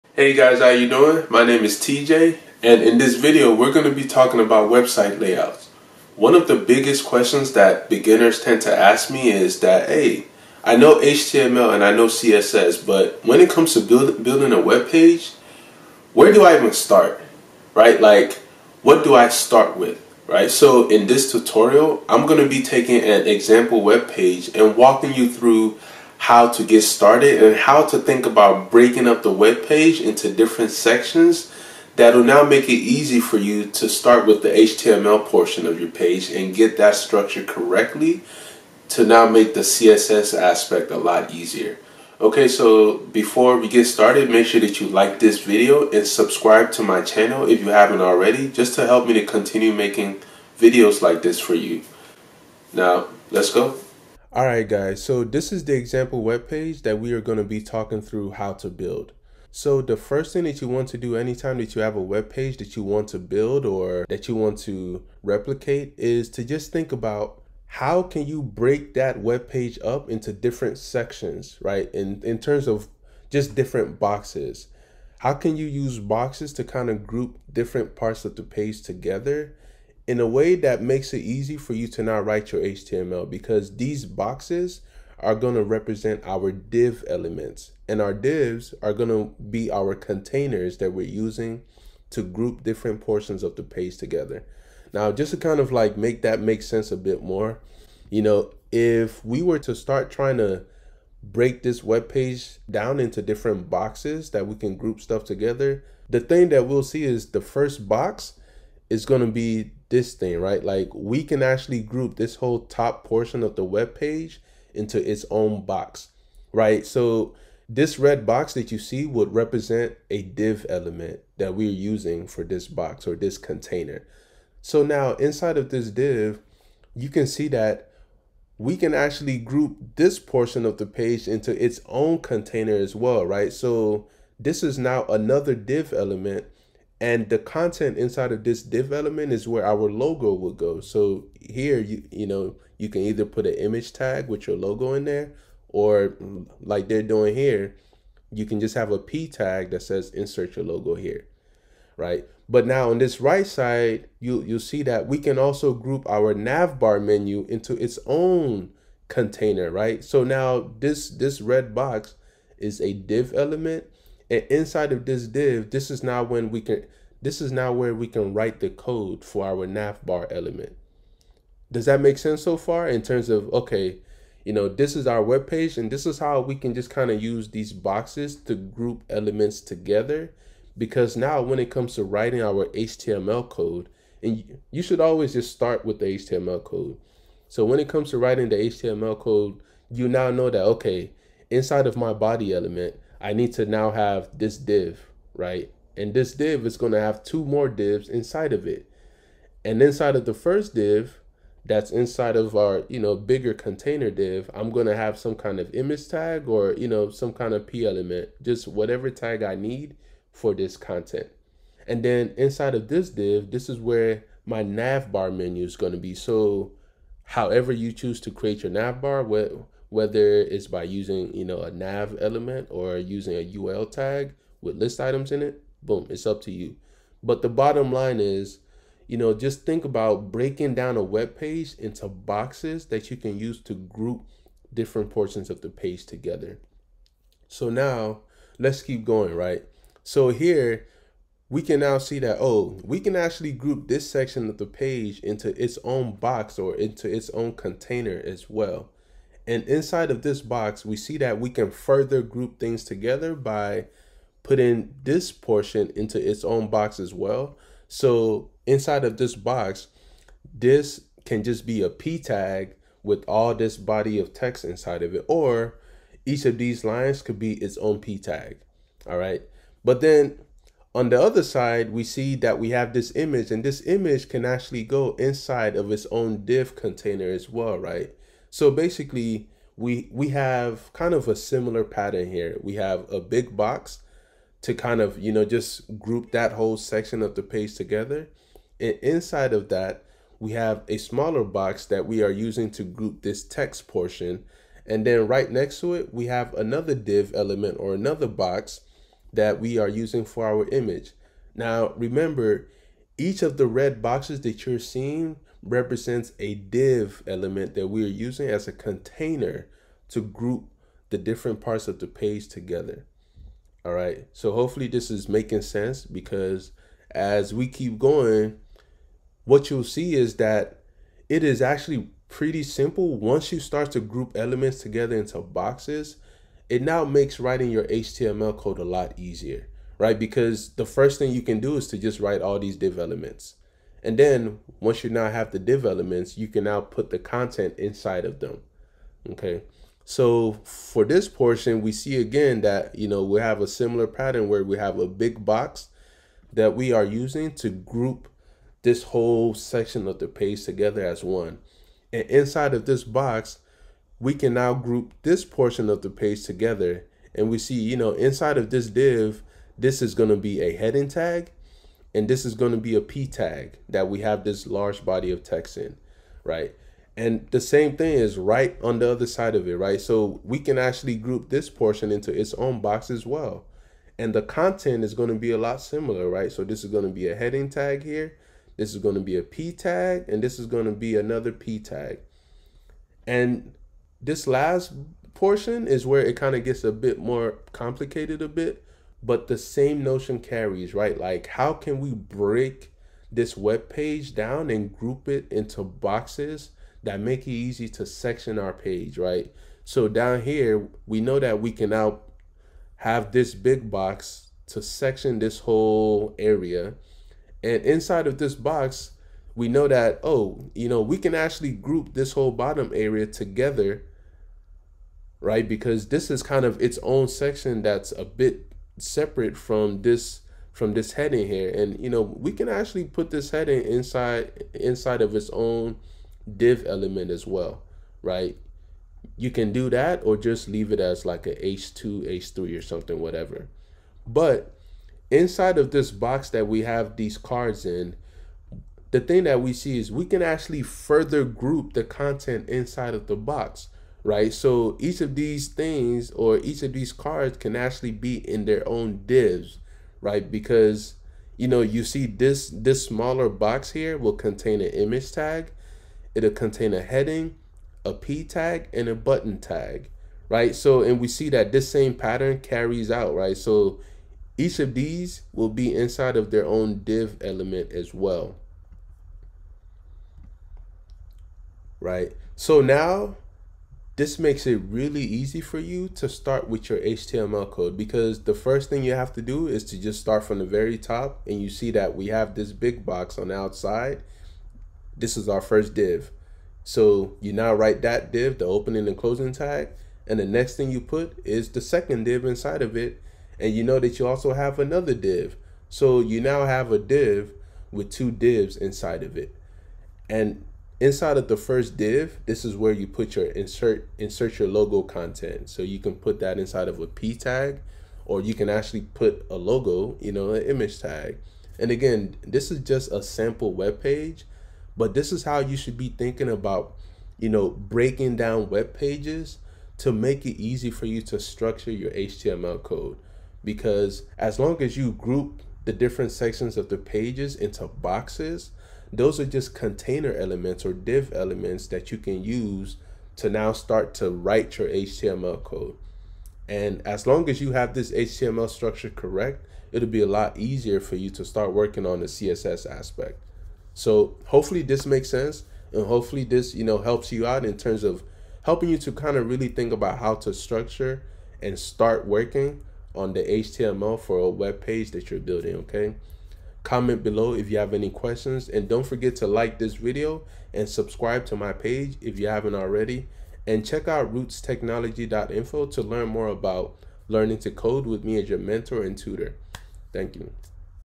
Learn to Code in 9 Minutes - Website Layout Tutorial | Structuring Your Website
When learning how to code, one of the biggest problems beginners have is not knowing how to get started when trying to build a website.

You're still getting the hang of HTML and CSS, and you're not sure how to even start, or what to start with. The HTML or the CSS?

So, in this video I'll be taking an example webpage and walking you through how to think about the layout and how to group the content so you can properly structure your HTML code. This will help make writing the CSS a lot easier and straightforward.